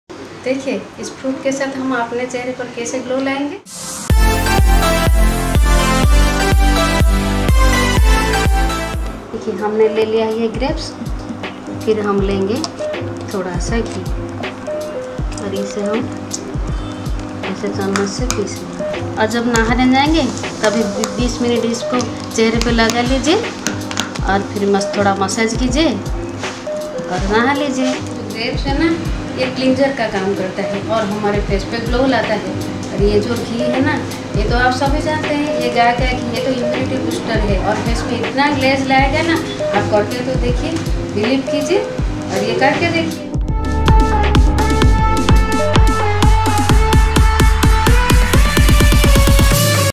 1 Minute Skincare Routine For Glowing Skin
Try this diy skincare routine made of grape and ghee.
Grape is filled with antioxidants and vitamin c . They have anti-ageing and skin vitalizing benefits.
Ghee is rich in omega 3,6 and 9.It has great hydrating and moisturizing properties. It is known to keep your face healthy and glowing.